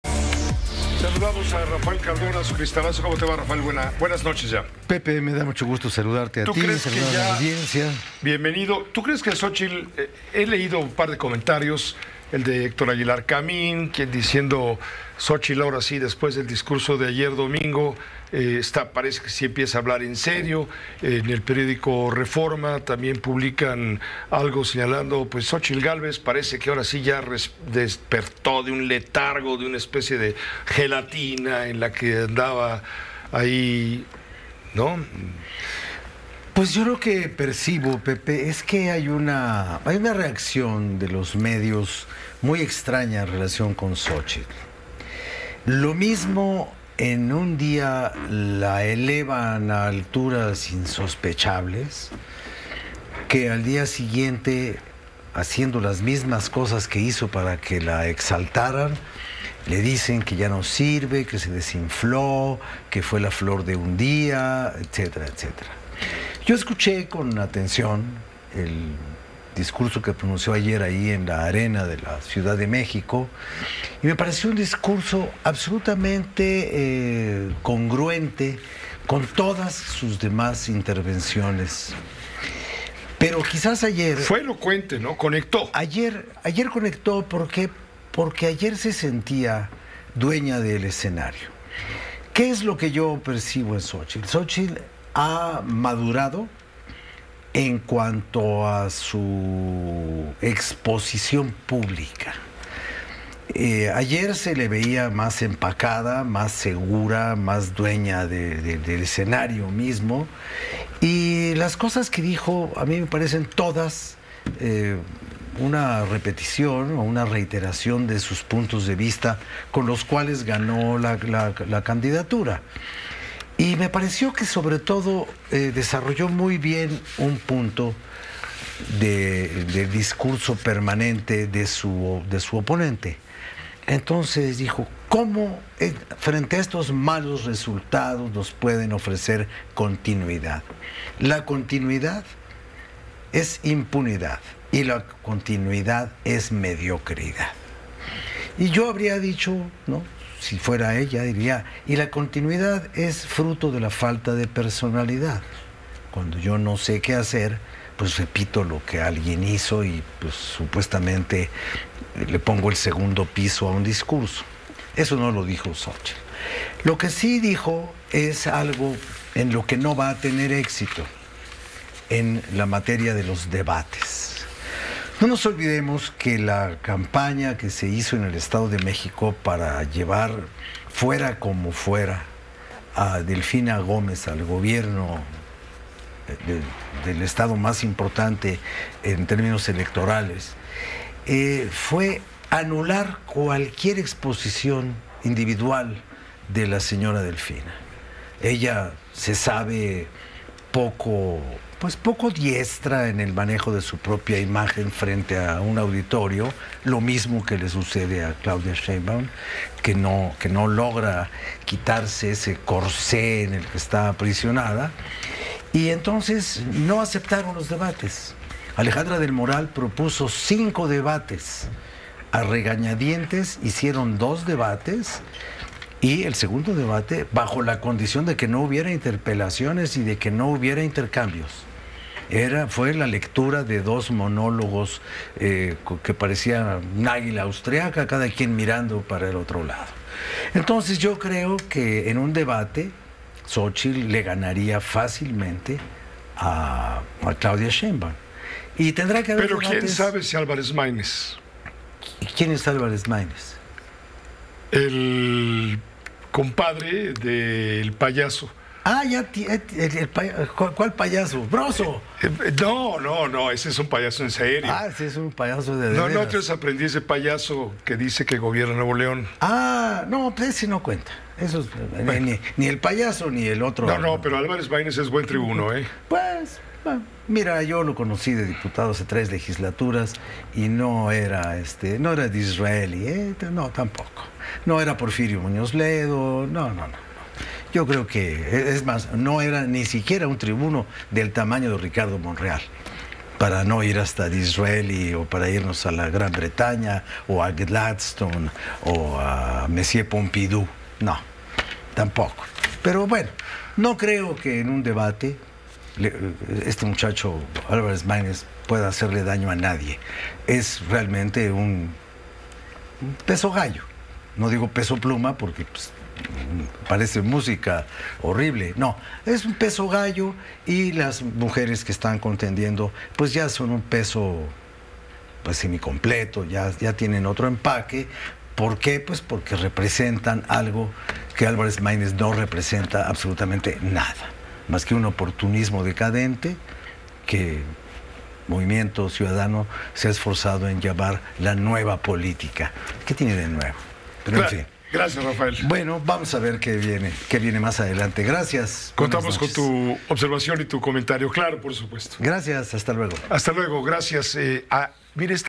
Xóchitl ganaría fácilmente en un debate a Claudia Sheinbaum: Rafael Cardona
Durante su cierre de precampaña este domingo, la precandidata a la presidencia de la República por el PAN, PRI y PRD, Xóchitl Gálvez Ruiz, aseguró que llegó el momento de luchar para traer la vida donde hoy hay muerte, verdad donde hoy reina la mentira, y libertad donde hoy gobierna el miedo.

Rafael Cardona analiza la actuación de Xóchitl en este cierre de precampaña, y asegura que conectó con los simpatizantes de la oposición porque se sintió dueña del escenario, y que este podría ser un fuerte para ella. También mencionó que los debates podrían ser otro fuerte a favor de Xóchitl, pero al igual que en la elecciones del Estado de México, se limitarán para evitar que Sheinbaum quede expuesta ante millones de mexicanos.

Grupo Fórmula